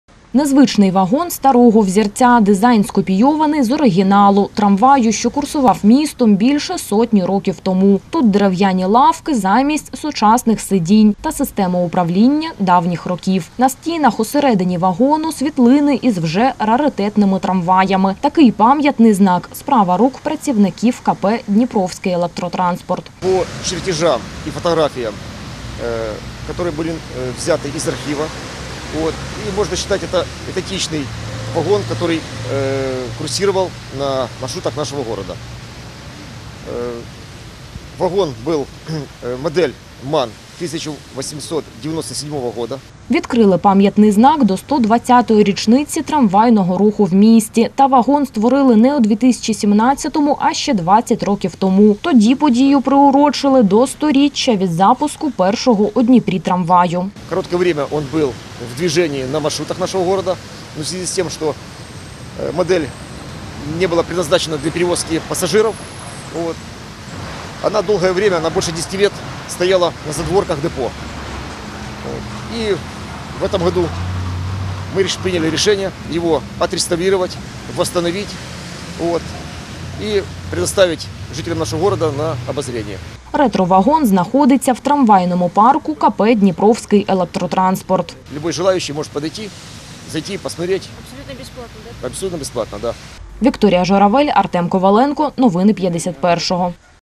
Раритетний трамвай
120 років трамвайному руху у Дніпрі. Таке свято відзначають 15-го грудня. 1897 році в Катеринославі розпочався рух електричного трамваю на трьох лініях : «Вокзал - Соборна», «Пристань-Базарна» та «Вокзал - Йорданська». Тоді містом курсували 14 вагонів, а трамвайне депо ще не побудували. Наразі ж такий вид електротранспорту став більш поширеним – в обласному центрі функціонують 14 трамвайних маршрутів, на них працюють 270 вагонів. З нагоди 120-річчя відкриття трамвайного руху в місті у Дніпрі відкрили пам’ятний знак – «Ретро трамвай». Це вагон, що відтворили за архівними кресленнями та фотографіями.